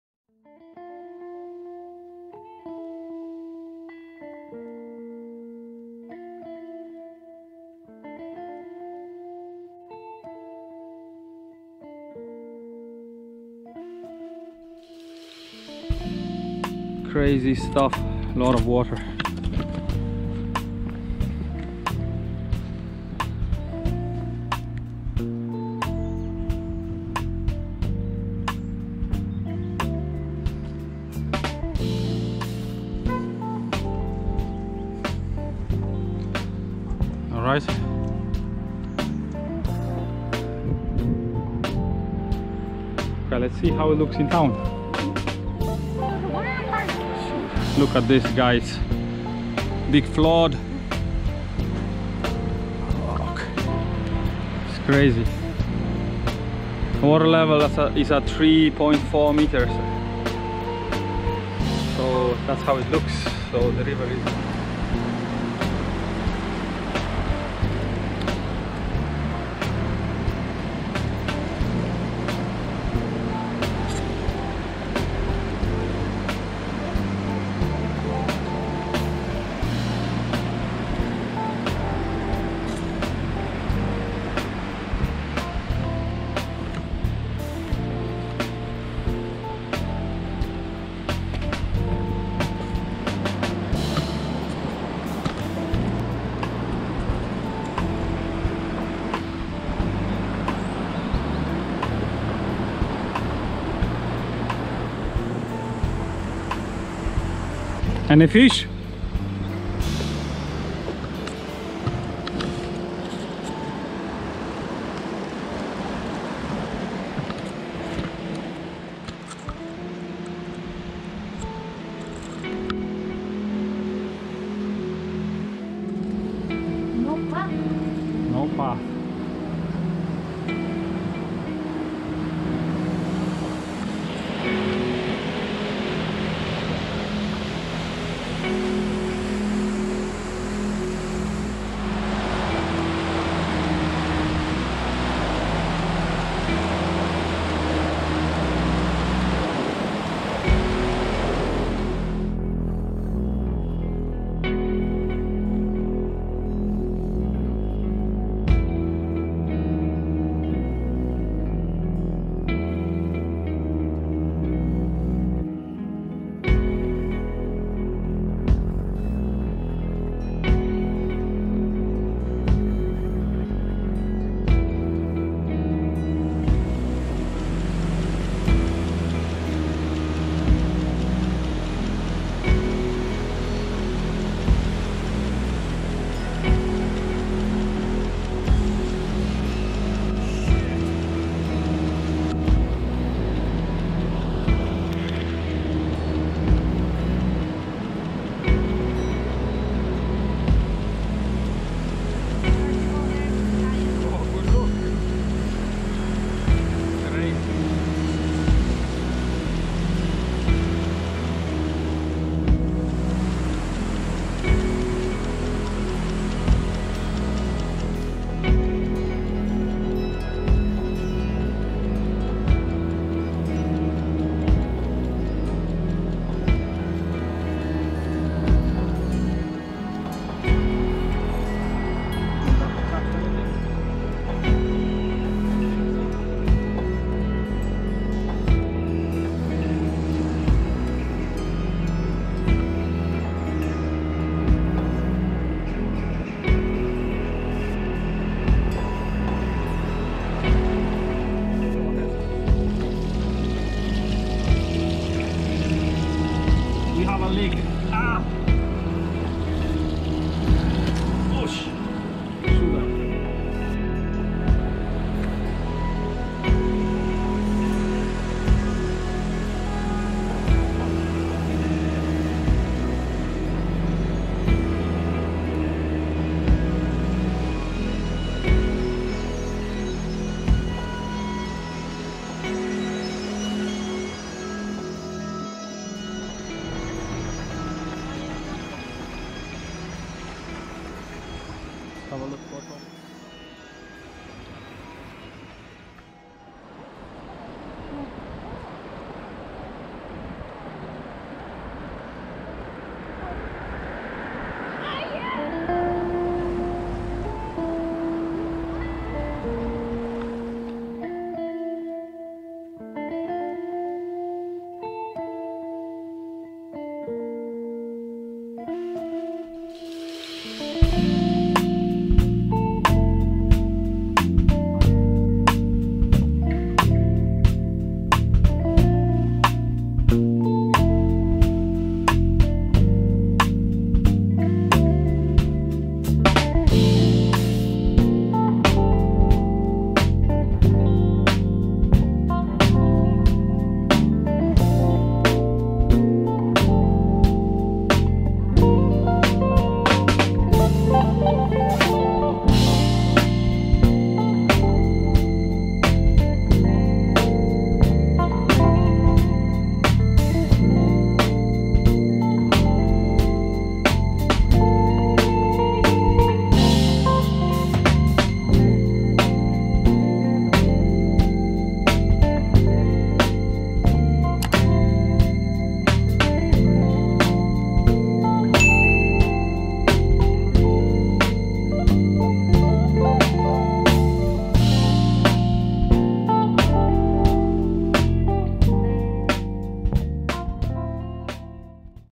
Clonmel flood 2021 - 3 bridges - 4K video
Recorded in Clonmel 24 Feb 2021
Walk from Presentation bridge to old bridge and Gashouse bridge.
Water level was at peak 3.4m.